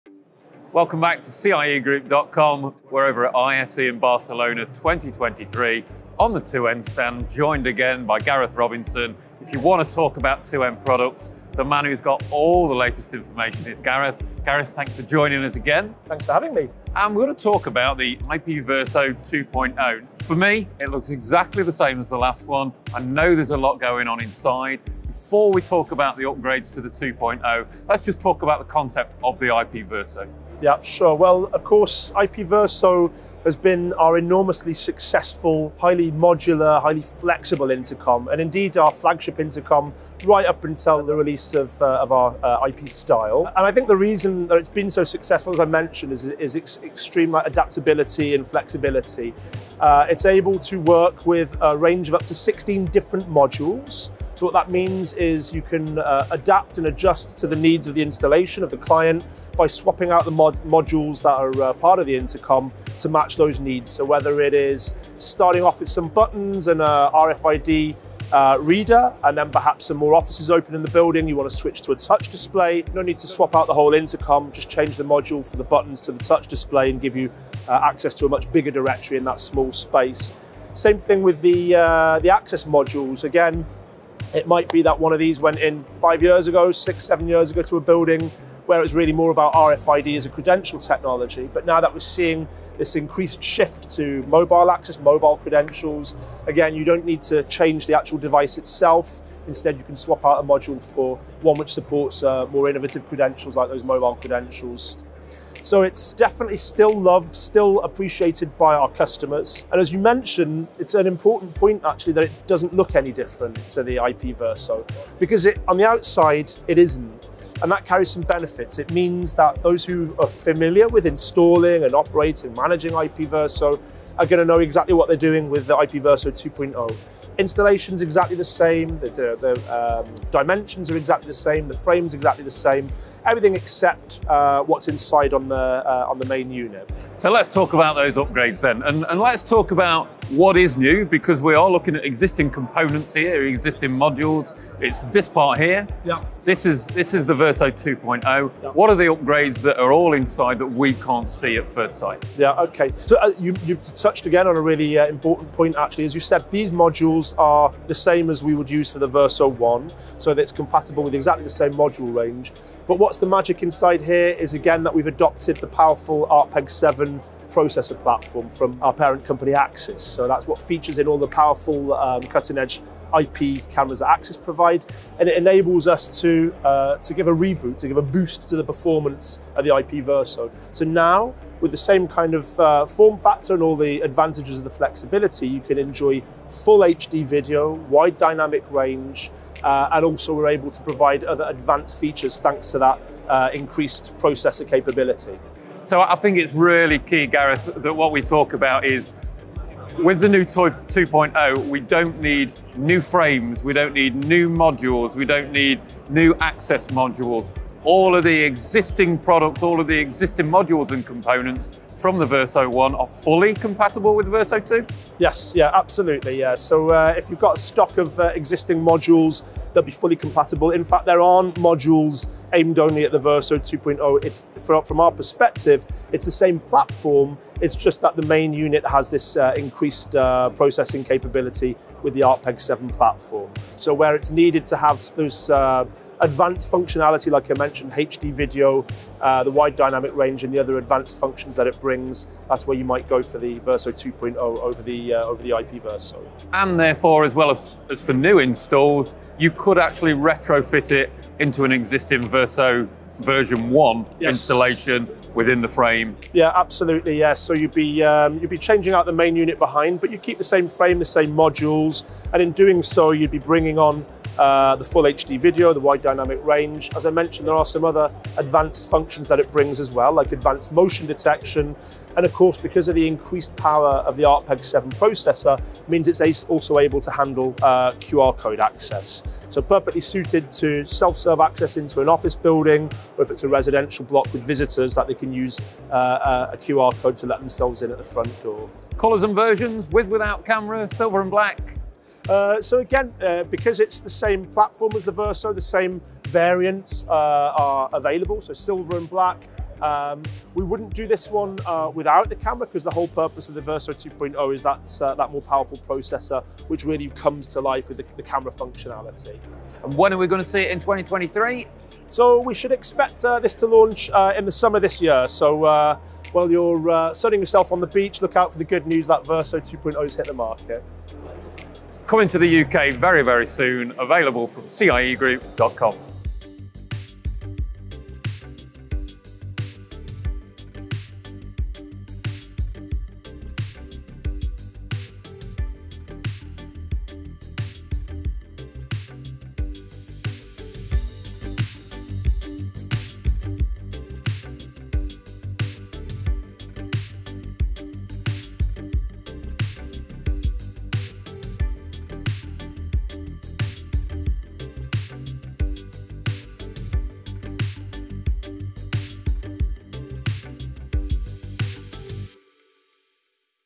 2N IP Verso 2.0 modular door intercom - new 2nd generation released
Introducing the second-generation of the market leading 2N Verso modular IP door intercom, Verso 2.0.
This cutting-edge device combines a wide-angle, Full HD camera and the latest ARTPEC-7 processor, enabling advanced functionalities such as QR code reading. With unmatched modularity, exceptional image quality and a diverse range of access methods, the 2N Verso 2.0 sets a new standard in door entry and access control technology.

Filmed on the @2nvideos booth at @IntegratedSystemsEurope

0:00 Introduction to 2N Verso 2.0
0:30 What is the concept of 2N IP Verso modular door intercom?
1:20 Modules for Mobile Access Credentials
2:16 What are the upgrades to new 2N Verso 2.0?
2:42 New AXIS ARTPEC7 processor
3:35 Modules compatible with original Verso and Verso 2.0
4:52 What versions will be available?